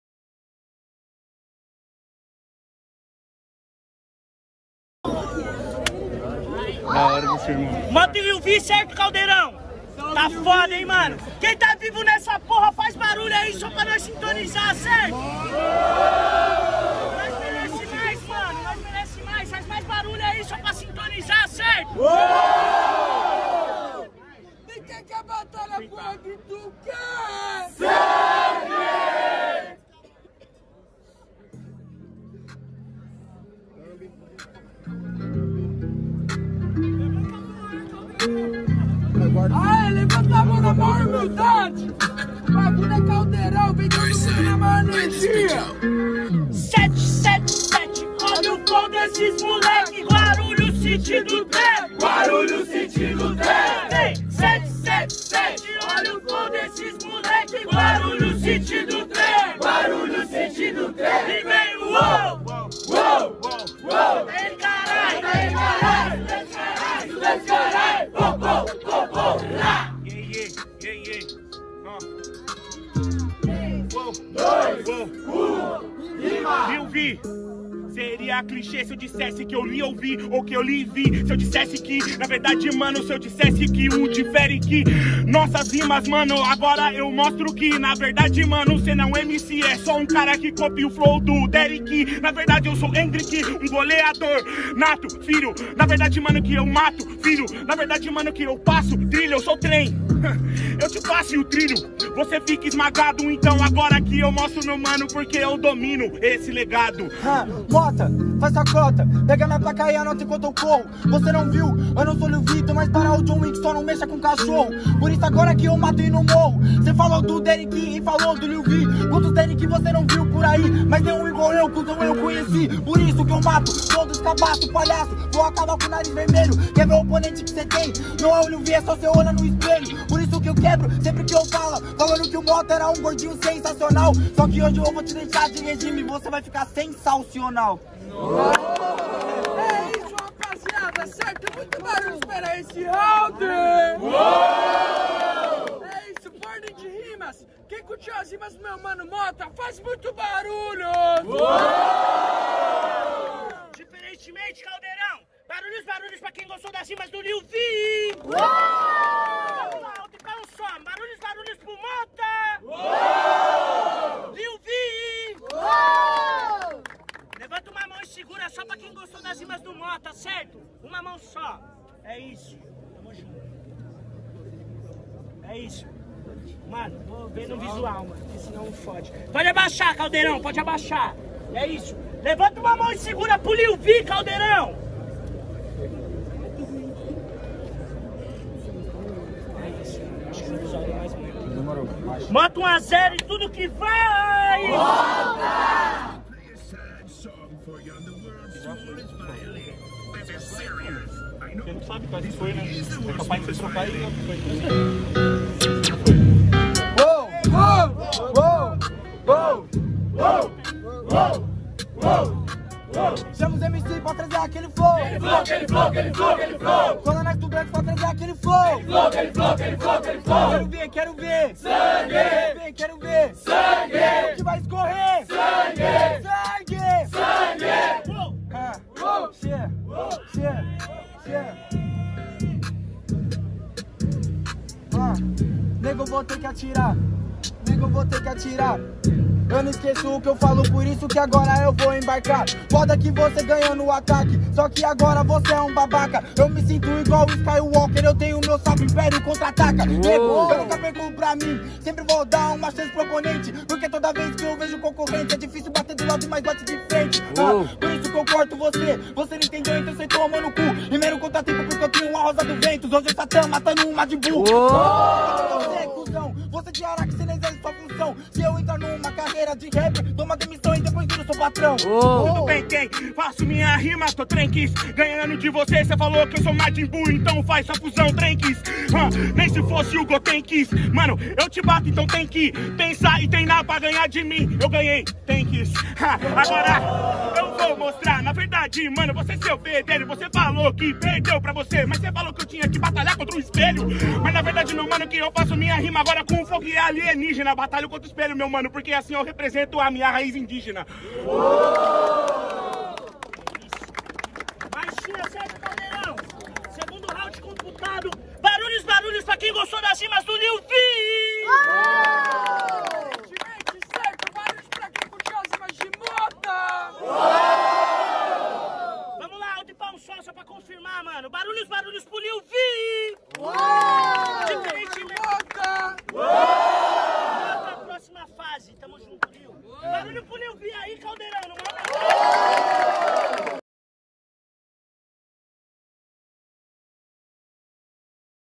(Chuvas de rimas 🎤) LIL VI x MOTTA | 1º FASE | 3º PARADA:PRAÇA DOS ESTUDANTES
2023 ITINERANTE -
1º PARADA : ROSSELVET
2º PARADA : LIBERDADE
3ºPARADA: PRAÇA DOS ESTUDANTES